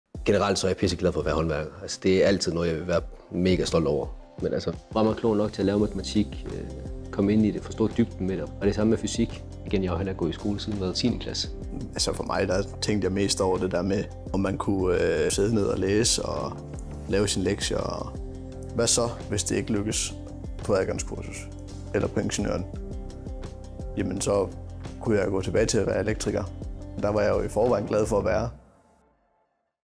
Er du håndværker, og overvejer du at læse til ingeniør?
Overvejer du at læse til ingeniør, men mangler du en adgangsgivende eksamen? Og har du bekymringer for et studieliv på SU, matematik på A-niveau, klassekammerater og alt muligt andet? Besøg os, og hør mere om studie og studieliv fra vores egne studerende, der selv er tidligere håndværkere. Hvis du gerne vil uddanne dig til ingeniør, men mangler en adgangsgivende eksamen – så kan du med Adgangskursus komme i gang efter bare ét år. Kurset giver dig alle nødvendige teoretiske forudsætninger for at læse videre, og det giver adgang til alle vores ingeniøruddannelser her på Aarhus Universitet. Du kan tage Adgangskursus i Aarhus, Herning eller endda online som fjernundervisningsforløb. Læs mere her: AK.AU.DK Du kan også finde kontaktinfo på vores studievejledere, der står klar til at hjælpe dig.